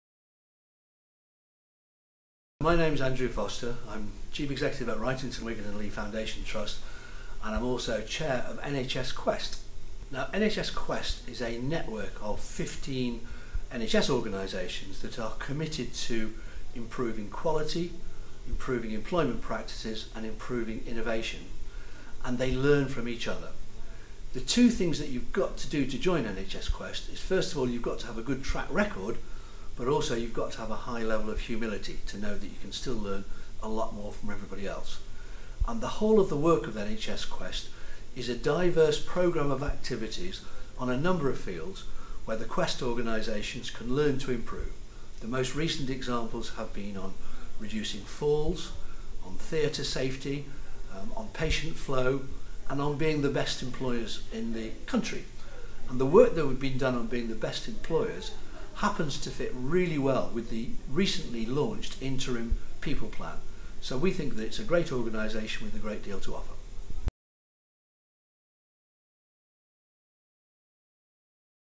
Andrew Foster introduces NHS Quest
Andrew Foster, Chief Executive of Wrightington, Wigan & Leigh NHS Foundation Trust and Chair of NHS Quest describes the work of the NHS Quest improvement programme and how the staff element of this dovetails with the interim NHS People strategy.